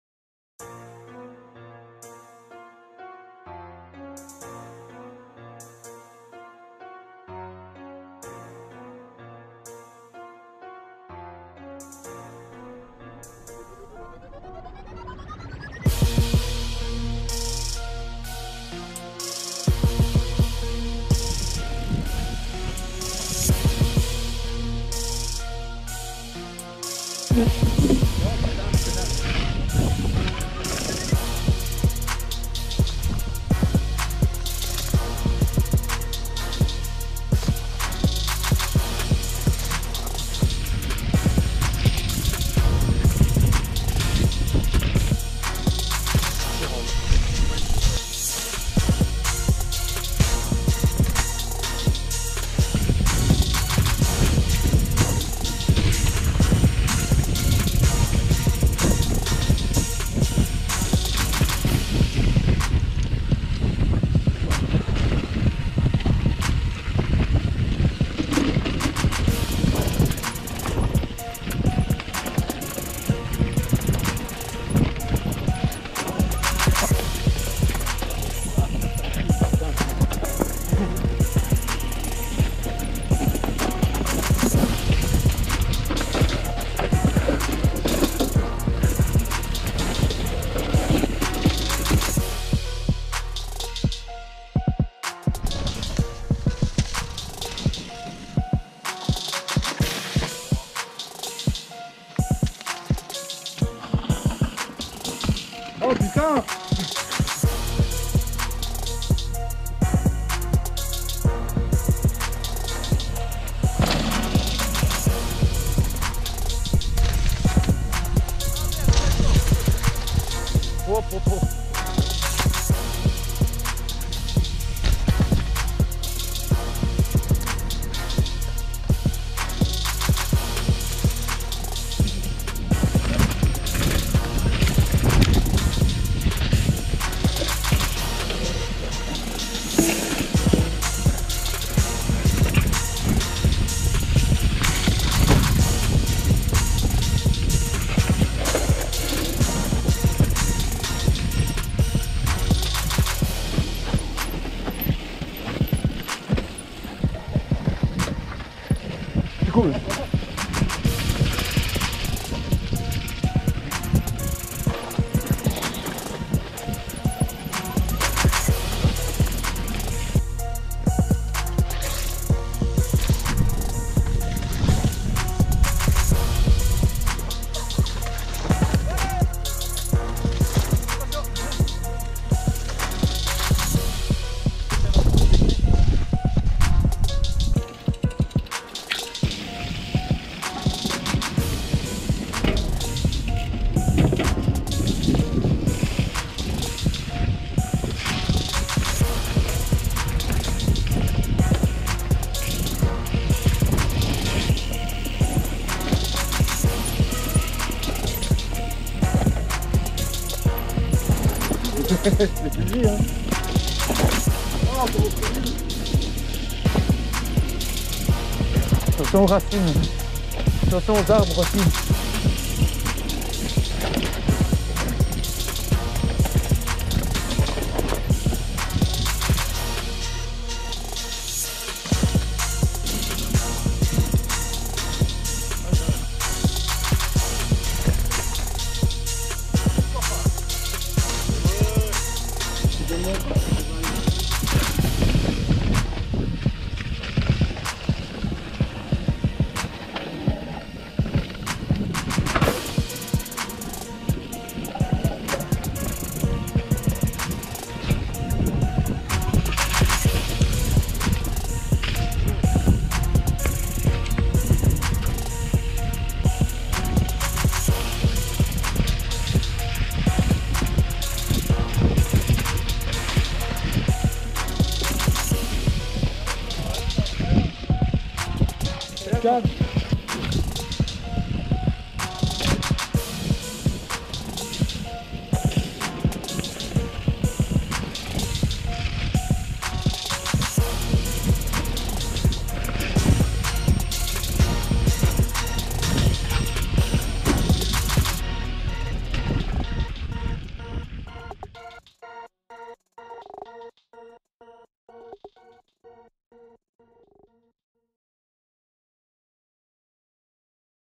ENDURO // BOIS DES REVES // VTT AVEC LES POTES
►Petite session au Bois des Rêves avec les potes.

►Materiel :
Gopro Hero 7 Black
Gopro Max